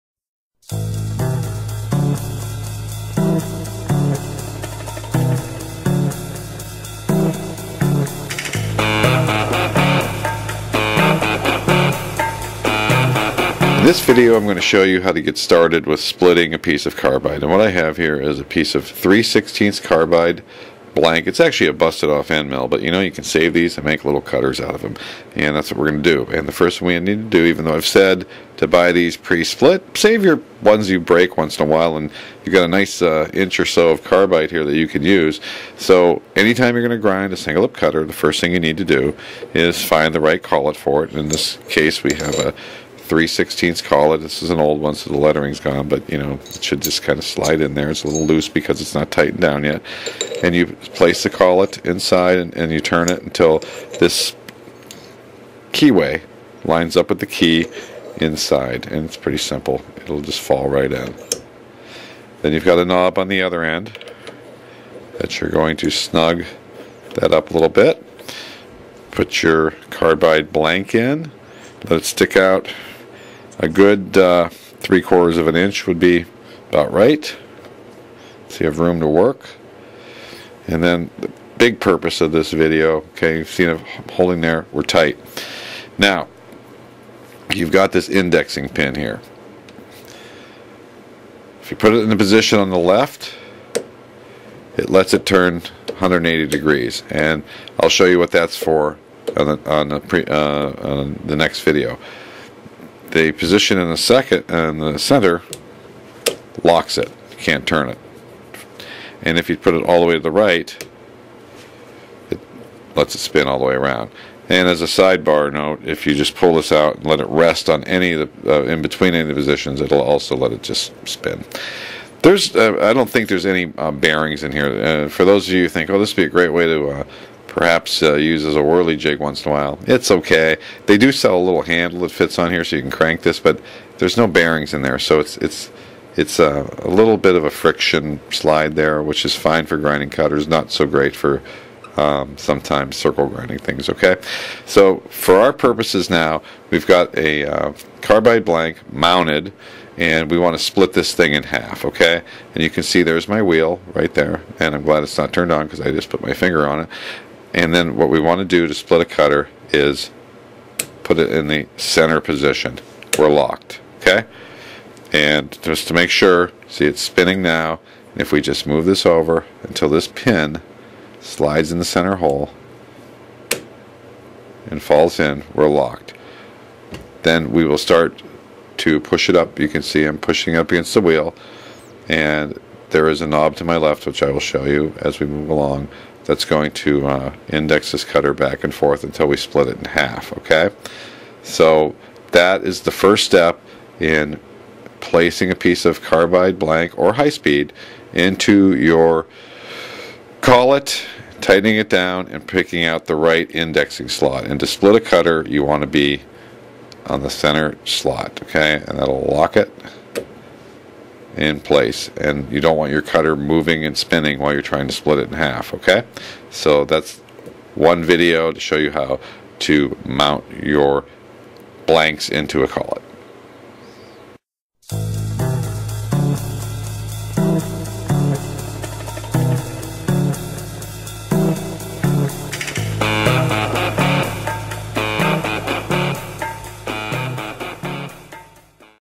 Deckel Lesson 3: Splitting a Carbide Blank
One of 10 complete set-up and operating instructions for the Deckel SO Cutter Grinder.